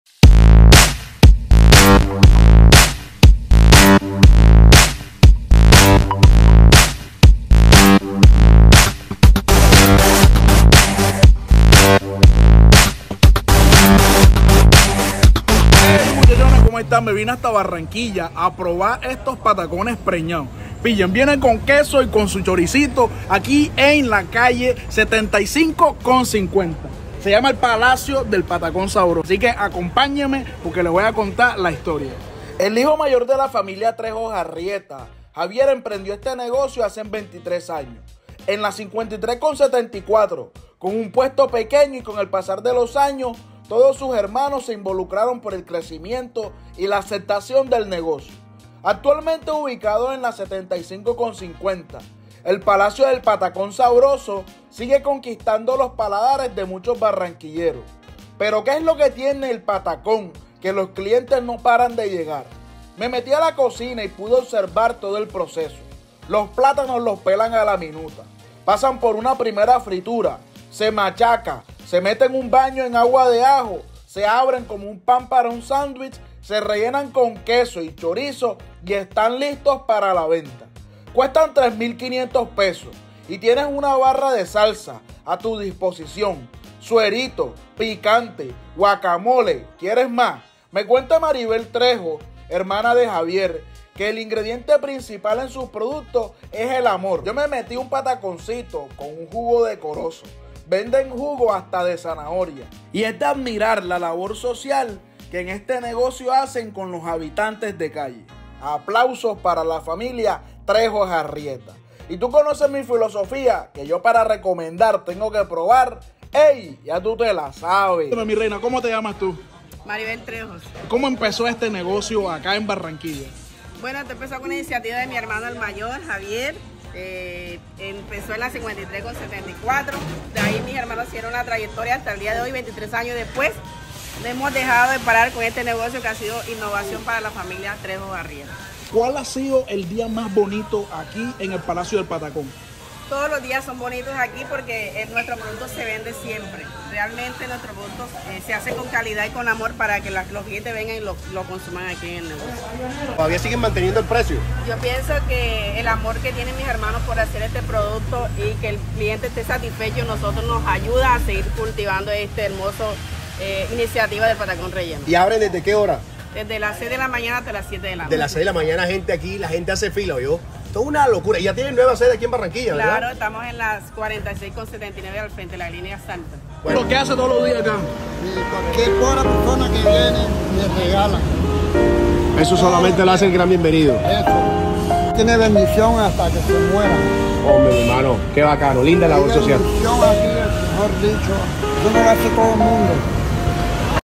El palacio del Patacón en Barranquilla
El palacio del patacón en Barranquilla, 23 años de tradición

Patacones hechos con amor con baño de ajo y rellenos de chorizo con queso que vaina plena.

Visítalos en 📍 75 con 50
Barranquilla, colombia

Horario 6pm a 6pm